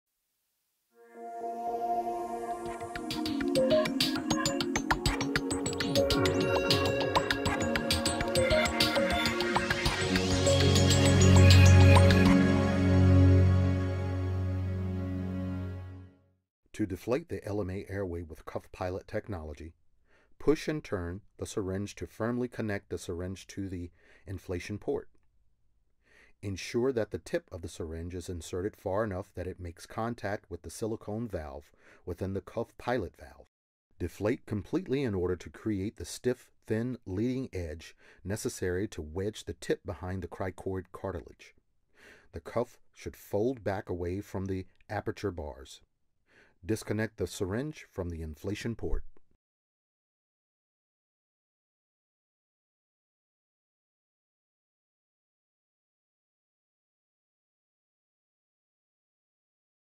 Cuff Pilot™ Technology - Pre-Insertion (video 5 of 6)
To deflate the LMA® Airway with Cuff Pilot™ Technology, push and turn the syringe to firmly connect a syringe to the inflation port. Ensure that the tip of the syringe is inserted far enough that it makes contact with the silicone valve within the Cuff Pilot™ valve.

Please refer to the package insert of the specific LMA® Airway product to be used for complete indications, contraindications, warnings, precautions, potential complications and Instructions for Use. MC 004607rev1